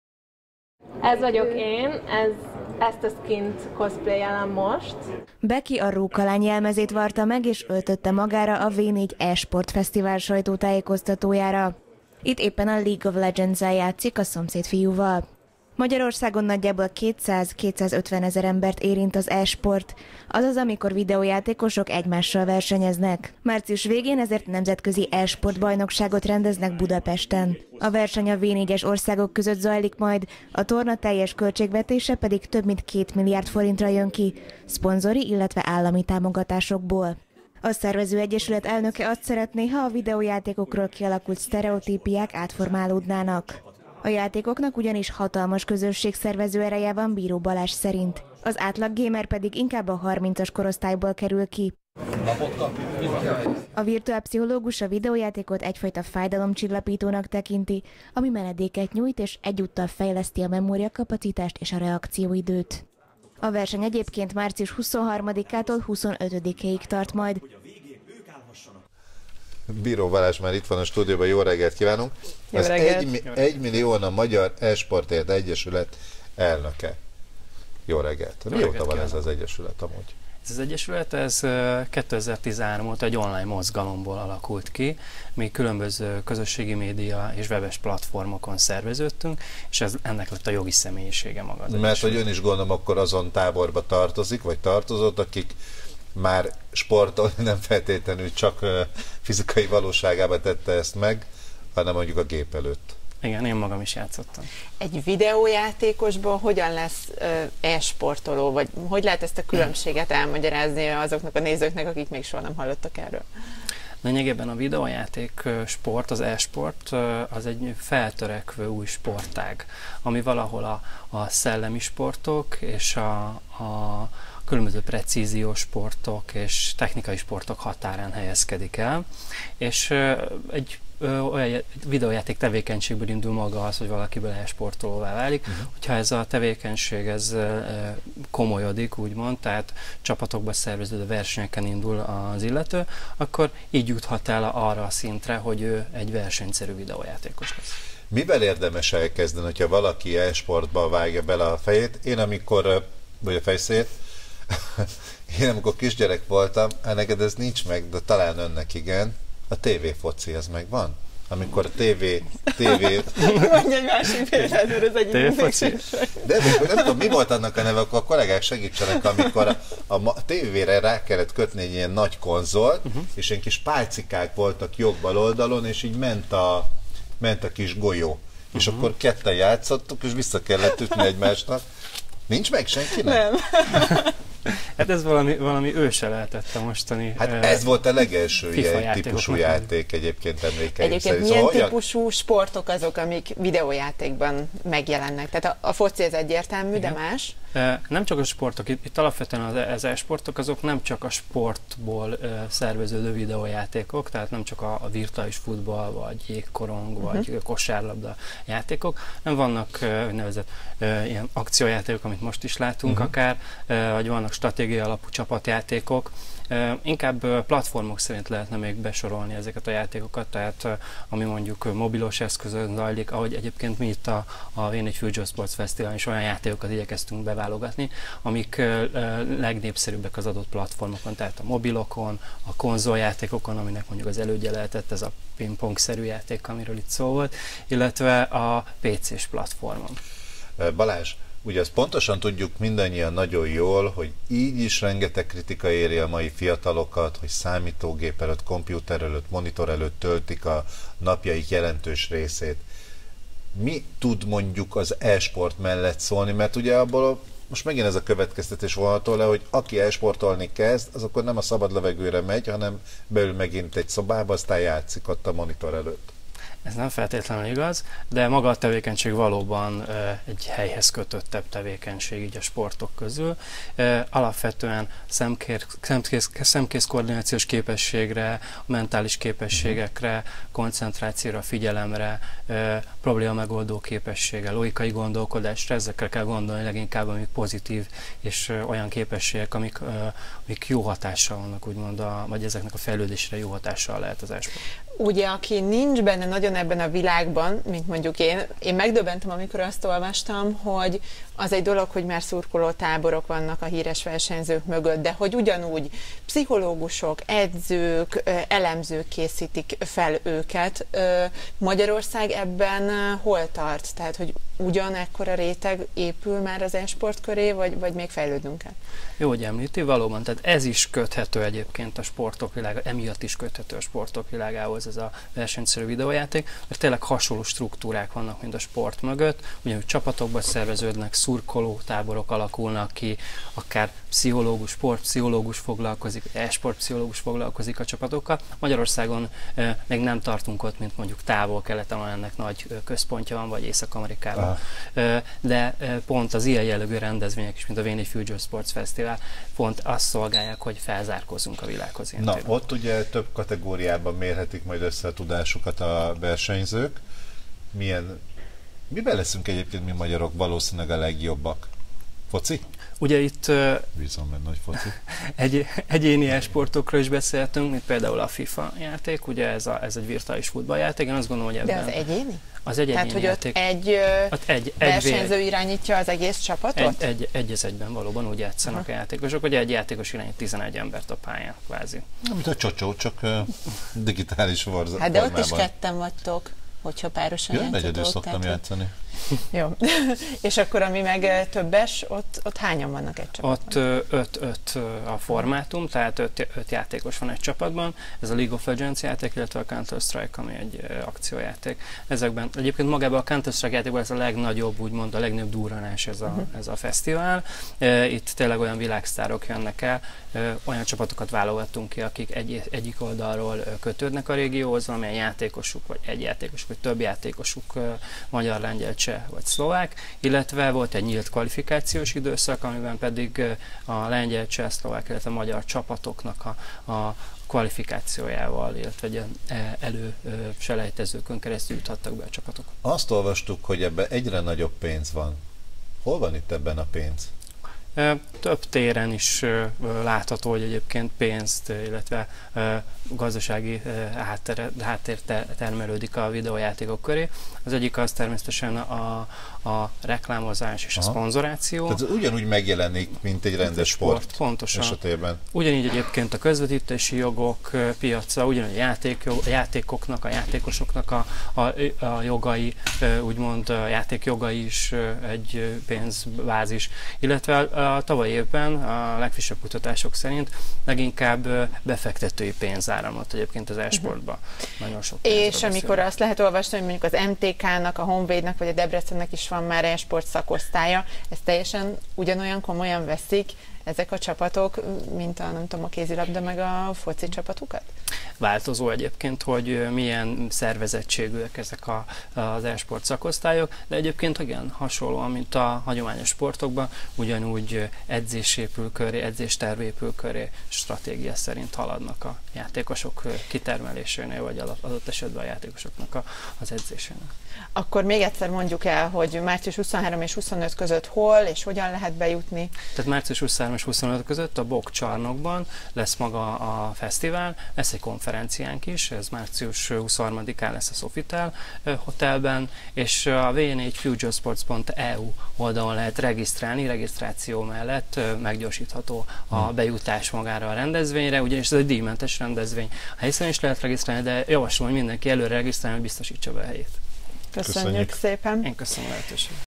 Kétmilliárdos támogatás a magyar e-sportnak
Az ATV Start vendége Biró Balázs elnök, Egymillióan a Magyar Esportért Egyesület.